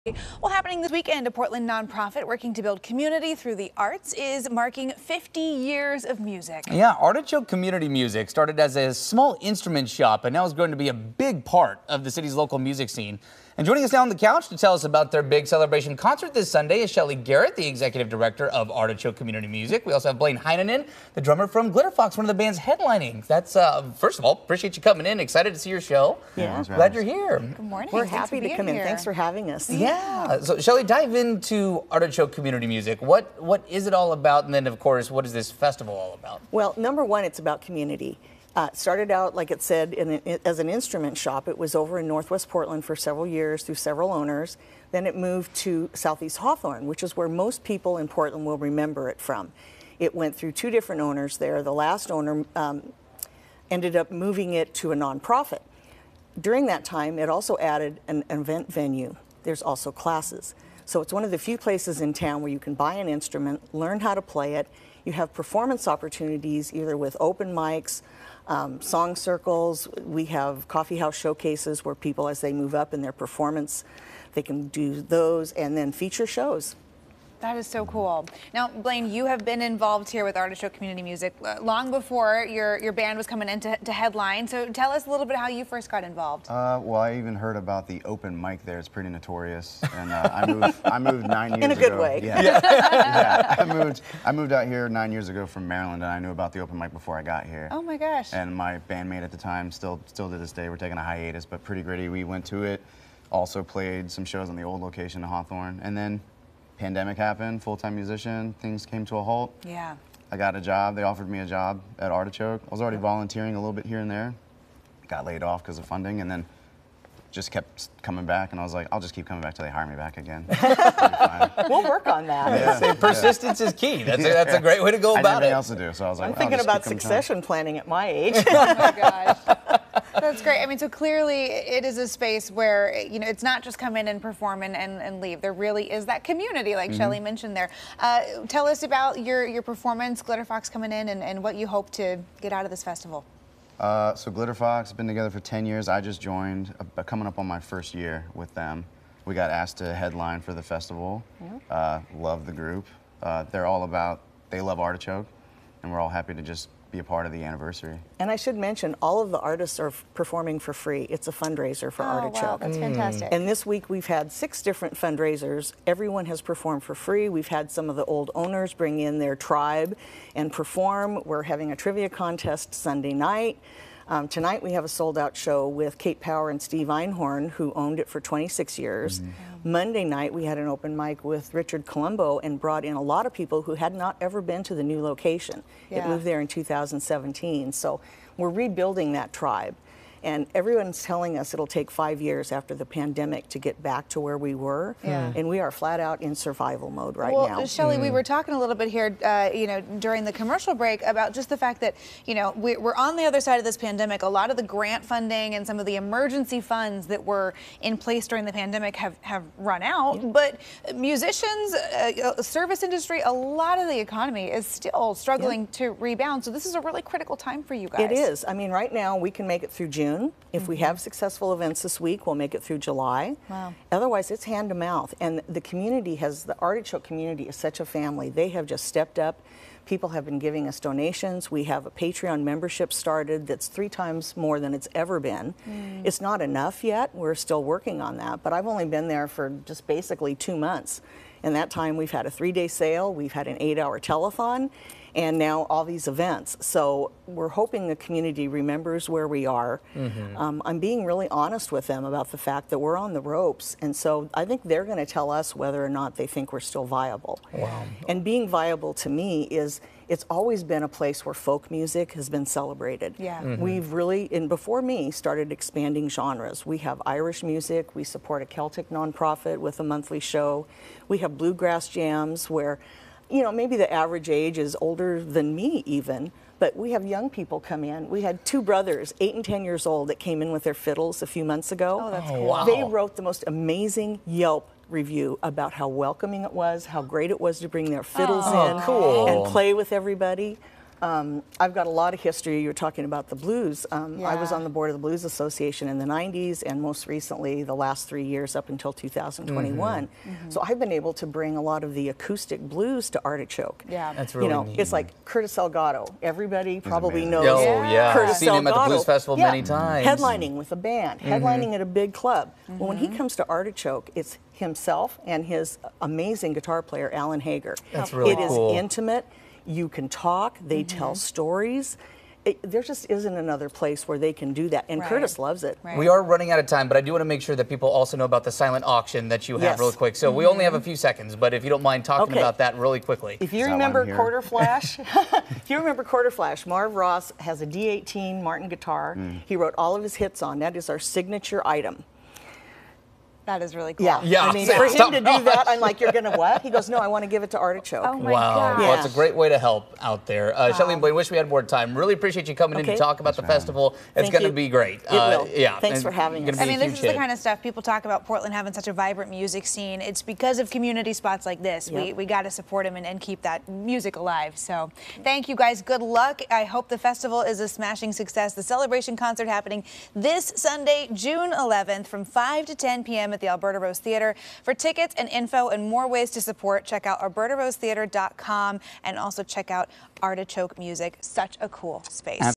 Artichoke Community Music holding 50th anniversary concert
Artichoke Community Music 50th Anniversary Concert Jue 11th 5pm at the Alberta Rose. TICKETS